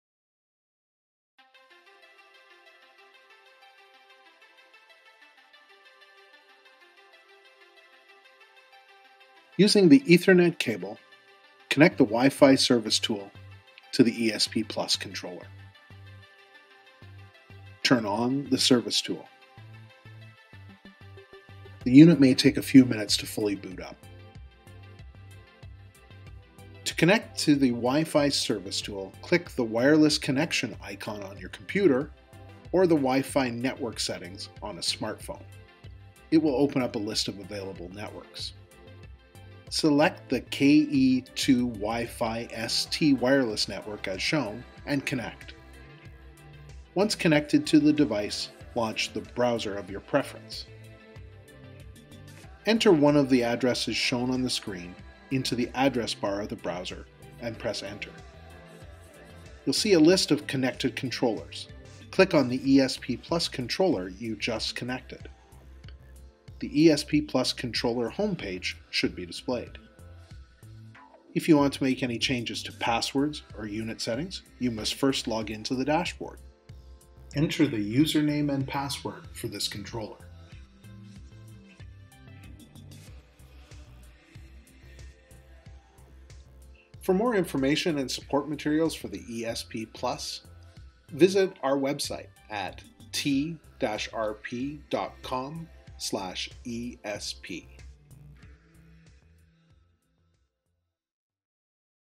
Video 9 - Connecting to ESP+ Using Wi-Fi Service Tool (Firmware V2.05)
Trenton Refrigeration's ESP+ intuitive evaporator control technology is designed to replace traditional electro-mechanical refrigeration controls typically used on medium and low-temperature applications.

Learn how to quickly connect to the ESP+ using the KE2 wi-fi service tool (optional) in this video